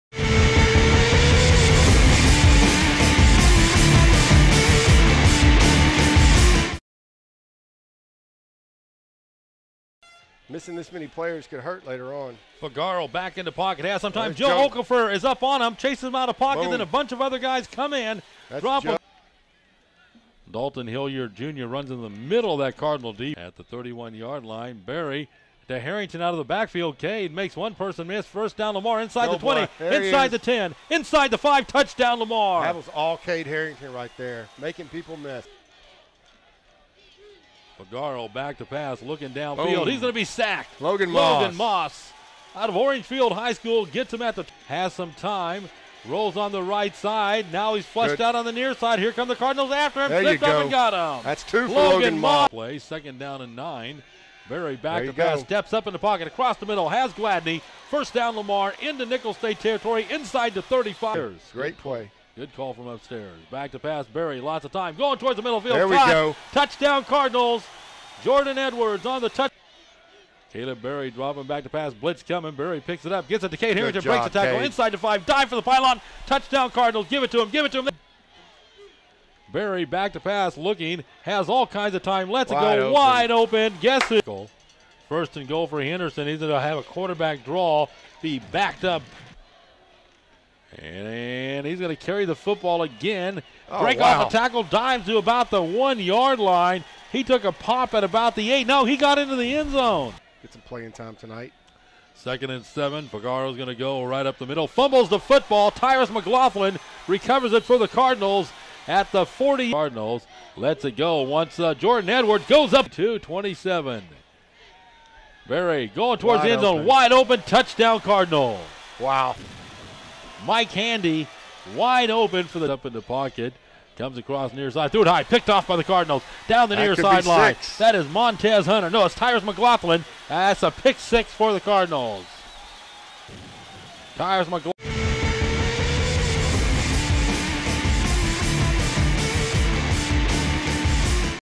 FOOTBALL: Nicholls State vs. Lamar University Highlights
Key moments from the Nicholls State vs. Lamar University game.